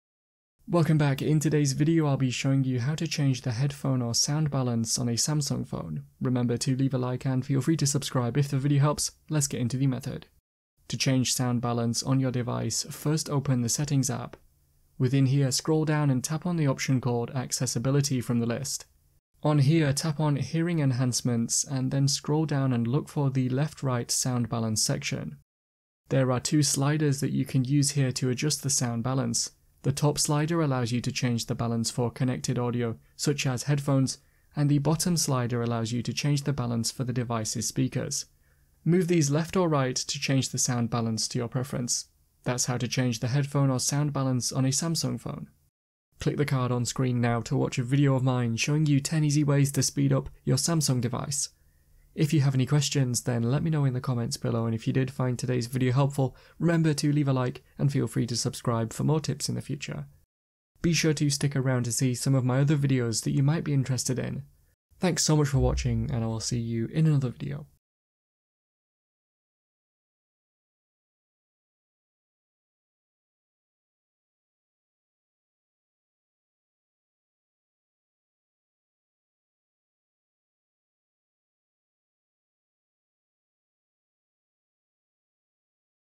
How to Change Headphone/Sound Balance on Samsung Phone
Want to know how to change sound balance on Samsung phone? This video will show you how to change headphone balance on Samsung phone. You might want to know how to change audio balance on Samsung phone make the left or right side louder. The method will work on all Samsung phones including Samsung Galaxy S23, Galaxy S24, Samsung Galaxy A series and more! Hopefully today’s video helps show you how to adjust sound balance on Samsung phone!